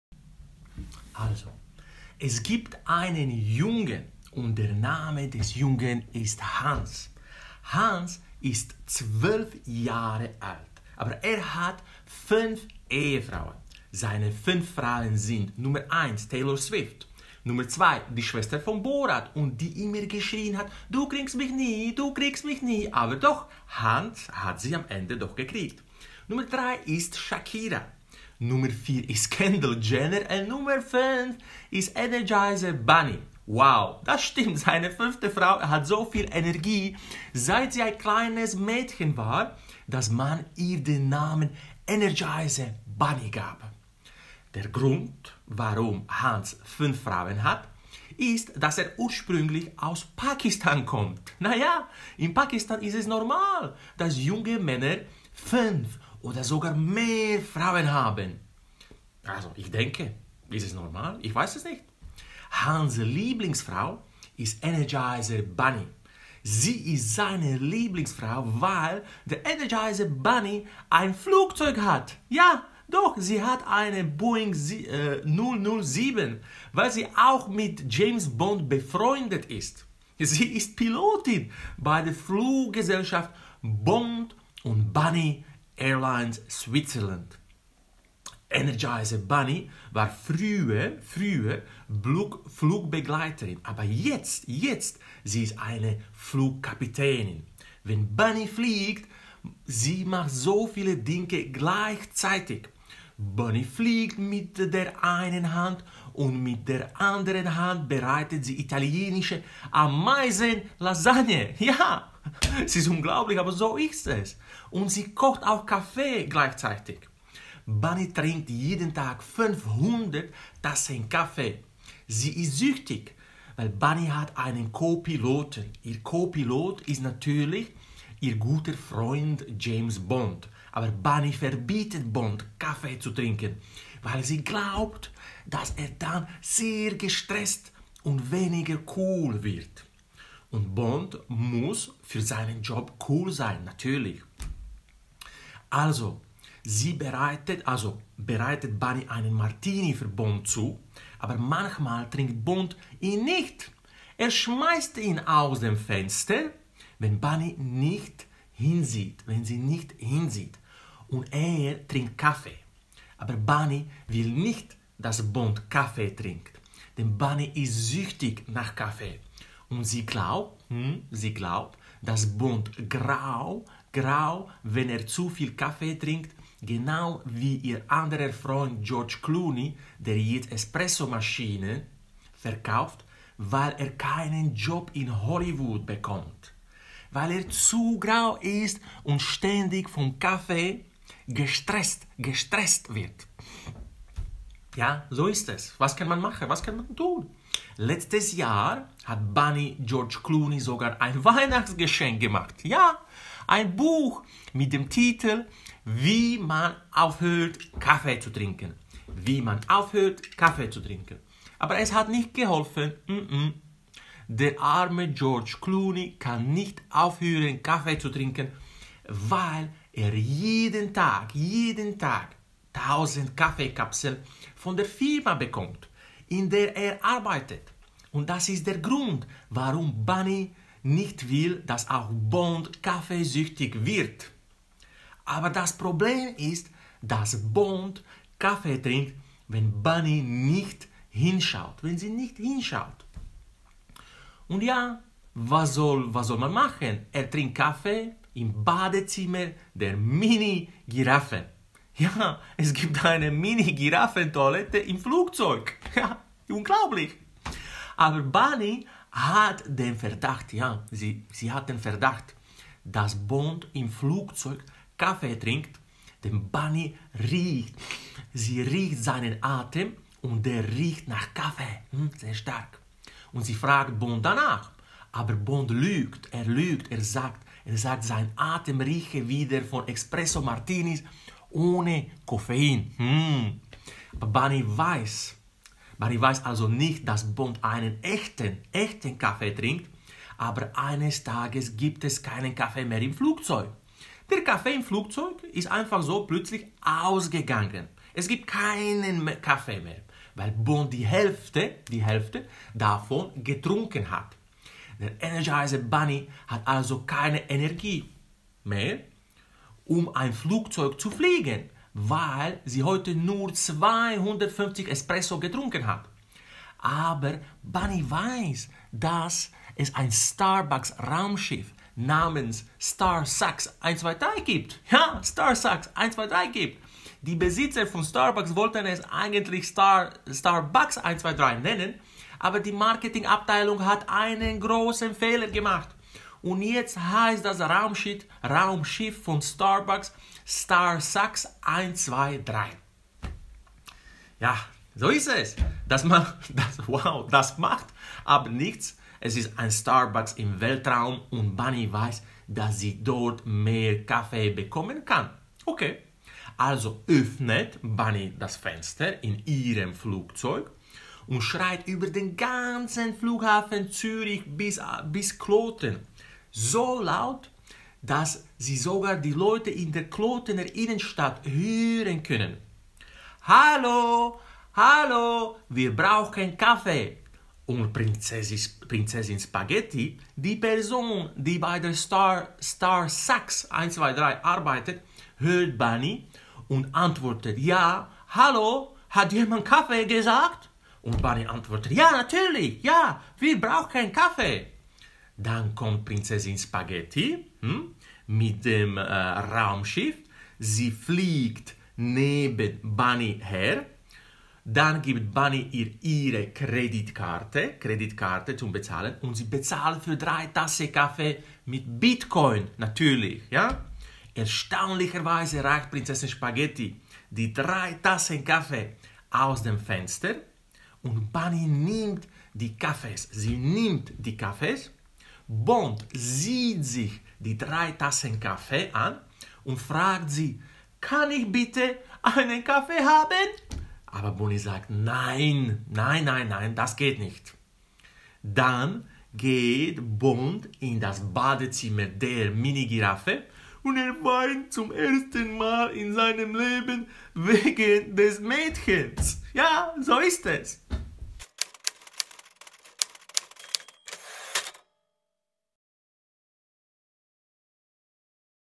Energizer Bunny Makes James Bond Cry - Intermediate (Crazy) German TPRS
Research and almost all the polyglots in the world show that the best way to learn a language is to have authentic experiences with it with Comprehensible Input.

Story-text:

Es gibt einen Jungen.  Der Name des Jungen ist Hans. Er ist 12 Jahre alt, aber er hat 5 Ehefrauen. Seine fünf Frauen sind Taylor Swift, die Schwester von Borat, die geschrien hat: "Du kriegst mich nie, du kriegst mich nie", aber Hans hat sie am Ende gekriegt... 3. ist Shakira, 4. ist Kendall Jenner und 5. ist Energizer Bunny.

Das stimmt, seine 5. Frau hat so viel Energie, seit sie ein kleines Mädchen war, dass man ihr den Namen Energizer Bunny gab.

Der Grund, warum Hans 5 Frauen hat, ist, dass er ursprünglich aus Pakistan kommt. In Pakistan ist es normal, dass junge Männer 5 oder mehr Frauen haben. Ich denke.

Hans' Lieblingsfrau ist Energizer-Häschen. Sie ist seine Lieblingsfrau, weil der Energizer-Hase ein Flugzeug hat. Sie hat eine Boeing 007.

Weil sie auch mit James Bond befreundet ist. Sie ist Pilotin bei der Fluggesellschaft - Bond and Bunny Airlines Switzerland. Energizer Bunny war früher Flugbegleiterin. Aber jetzt ist sie ein Flugkapitän. Wenn Bunny fliegt, macht sie viele Dinge gleichzeitig. Bunny fliegt mit der einen Hand und mit der anderen Hand bereitet sie italienische Ameisen Lasagne zu und kocht Kaffee.

Bunny trinkt jeden Tag 500 Tassen Kaffee.  Sie ist süchtig.  Bunny hat einen Co-Piloten.  Ihr Co-Pilot ist natürlich ihr guter Freund James Bond. Aber Bunny verbietet Bond, Kaffee zu trinken, weil sie glaubt, dass er dann gestresst und weniger cool ist.

Und Bond muss für seinen Job cool sein.

Also bereitet Bunny einen Martini für Bond zu, aber manchmal trinkt Bond ihn nicht. Er schmeißt ihn aus dem Fenster, wenn Bunny nicht hinsieht, und trinkt Kaffee.

Aber Bunny will nicht, dass Bond Kaffee trinkt, denn Bunny ist süchtig nach Kaffee und sie glaubt, dass Bond grau wird, wenn er zu viel Kaffee trinkt, genau wie ihr anderer Freund George Clooney, der jetzt Espressomaschinen verkauft, weil er keinen Job in Hollywood bekommt, weil er zu grau ist und ständig vom Kaffee gestresst wird.

Letztes Jahr hat Bunny George Clowney sogar ein Weihnachtsgeschenk gemacht, ein Buch mit dem Titel "Wie man aufhört, Kaffee zu trinken". Aber es hat nicht geholfen. Der arme George Clowney kann nicht aufhören, Kaffee zu trinken, weil er jeden Tag 1000 Kaffeekapseln von der Firma bekommt, in der er arbeitet. Und das ist der Grund, warum Bunny nicht will, dass auch Bond kaffeesüchtig wird.

Aber das Problem ist, dass Bond Kaffee trinkt, wenn Bunny nicht hinschaut... Ja, was soll man machen?

Er trinkt Kaffee im Badezimmer der Mini-Giraffe. Es gibt eine Mini-Giraffen-Toilette im Flugzeug.

Aber Bunny hat den Verdacht, dass Bond im Flugzeug Kaffee trinkt, denn Bunny riecht seinen Atem und der riecht nach Kaffee. Sie fragt Bond danach? Aber Bond lügt. Er sagt, sein Atem rieche wie der von Expresso Martinis ohne Koffein.

Bunny weiß also nicht, dass Bond einen echten Kaffee trinkt.  Aber eines Tages gibt es keinen Kaffee mehr im Flugzeug. Der Kaffee im Flugzeug ist ausgegangen. Es gibt keinen Kaffee mehr, weil Bond die Hälfte davon getrunken hat. Der Energizer-Hase hat also keine Energie mehr, um ein Flugzeug zu fliegen, weil er heute nur 250 Expressos getrunken hat.

Aber Bunny weiß, dass es ein Starbucks-Raumschiff namens 'Star-sucks 123' gibt.Die Besitzer von Starbucks wollten es 'Starbucks-123' nennen, aber die Marketingabteilung hat einen Fehler gemacht, und jetzt heißt das Raumschiff von Starbucks Star-sucks 123.

Das macht aber nichts. Es ist ein Starbucks im Weltraum und Bunny weiß, dass sie dort mehr Kaffee bekommen kann.
Also öffnet Bunny das Fenster in ihrem Flugzeug und schreit über den ganzen Flughafen Zürich. So laut, dass sie sogar die Leute in der Klotener Innenstadt hören können.

"Hallo, wir brauchen Kaffee", und Prinzessin Spaghetti, die Person, die bei Star-sucks 123 arbeitet, hört Bunny zu und antwortet: "Ja, hallo, hat jemand Kaffee gesagt?" und Bunny antwortet: "Ja, wir brauchen Kaffee." Dann kommt Prinzessin Spaghetti mit dem Raumschiff an und fliegt neben Bunny her.  Dann gibt Bunny ihr ihre Kreditkarte, und sie bezahlt für 3 Tassen Kaffee mit Bitcoin.

Erstaunlicherweise reicht Prinzessin Spaghetti die drei Tassen Kaffee aus dem Fenster und Bunny nimmt die Kaffees. Bond sieht sich die drei Tassen Kaffee an und fragt sie:

 -Kann ich einen Kaffee haben?

 -NEIN", antwortet Bunny.

Dann geht Bond in das Badezimmer der Mini-Giraffe und weint zum ersten Mal in seinem Leben wegen des Mädchens.